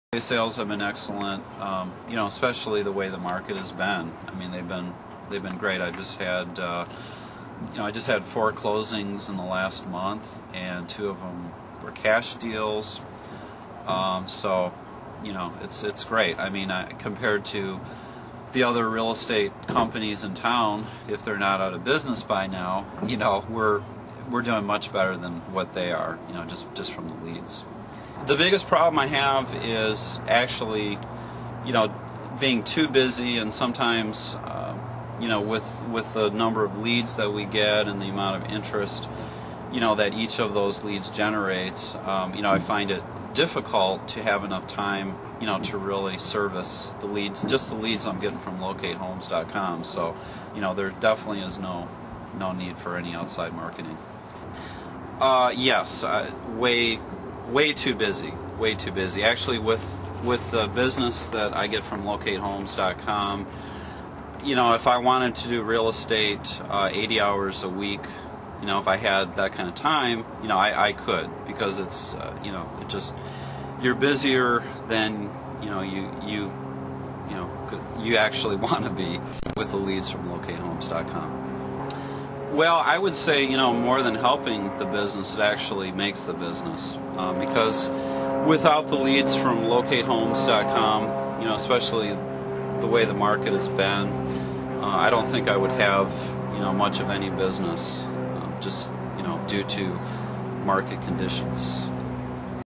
Keith Christensen, REALTOR - Testimonial
Keith Loves how it has helped him in a slow market